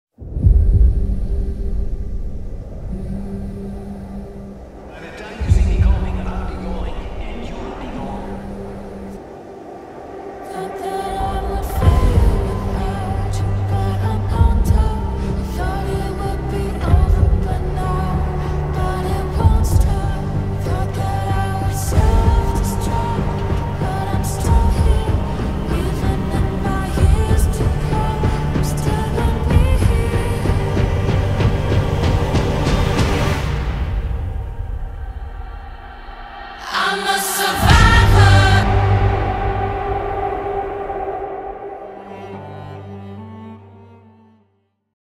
Octane: An Apex Story - Fan Made 3D Animation
On my path to try and learn how to use Blender and this is my first teaser type animation using Apex models featuring Octane.